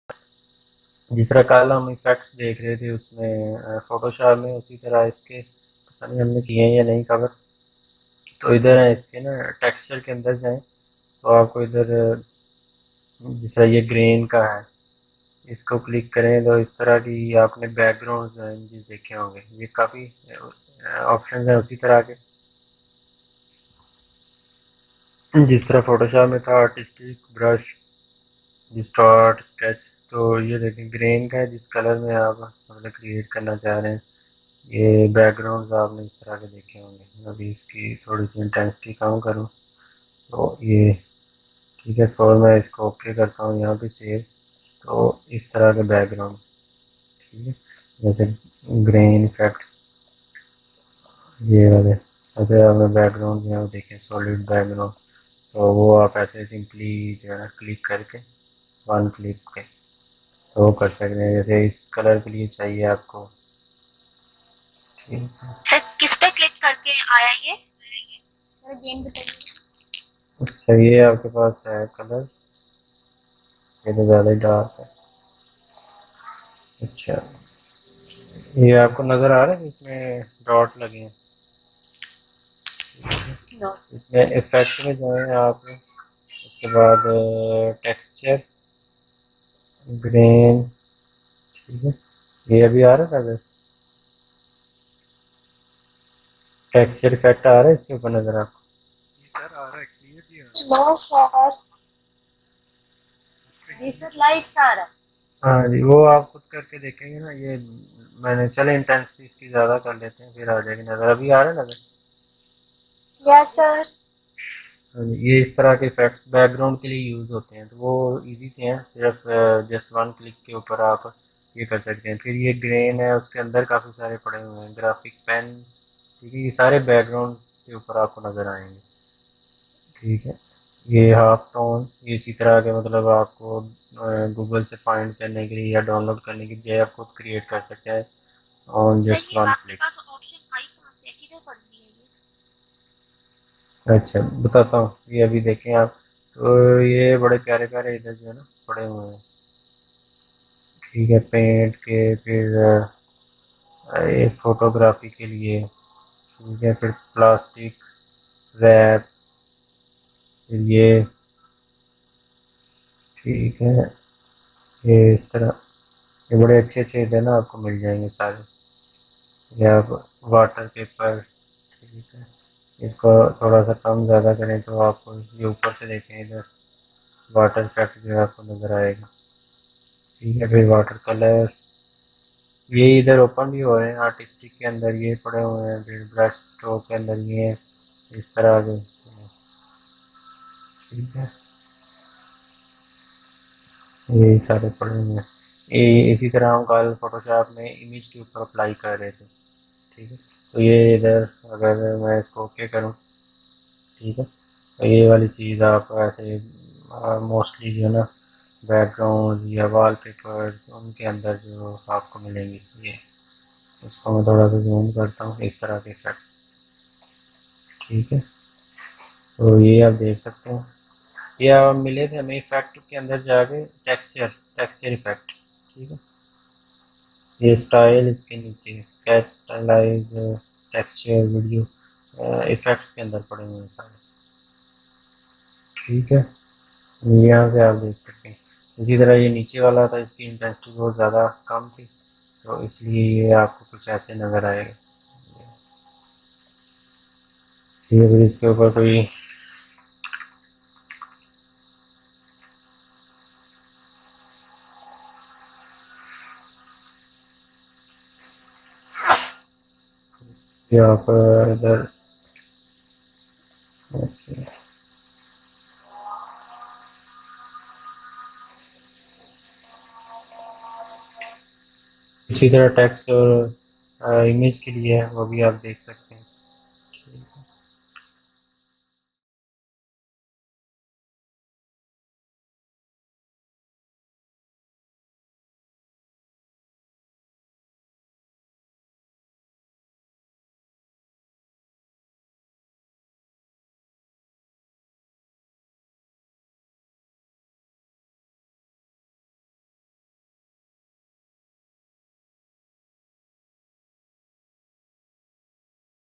Adobe Illustrator Lesson 16th NOISE GRAIN TEXTURE without brushes Illustrator tutorial Urdu / Hindi
This video is easy and quick to learn for beginners and intermediate graphic designers looking to improve their skills in Adobe Illustrator CC using some of my tips and tricks.

You will learn step by step how to import a sketch into Illustrator. Then set it as a template. We will then create the main outlines using the pen tool, shape builder tool, curvature tool, pathfinder when merging as well as how I choose my colors and gradients with my custom color palette.

► VIDEO GUIDE:
0:00 Intro
0:12 1st way: add textures using opacity mask and linear gradient
01:23 2nd way: imitate a noise brush using opacity mask and freeform gradient
03:01 3d way: learn how to create multilayered textures with rim light, shadows, and different settings to bring your illustrations to the next level
05:35 bonus tip on how to control texture resolution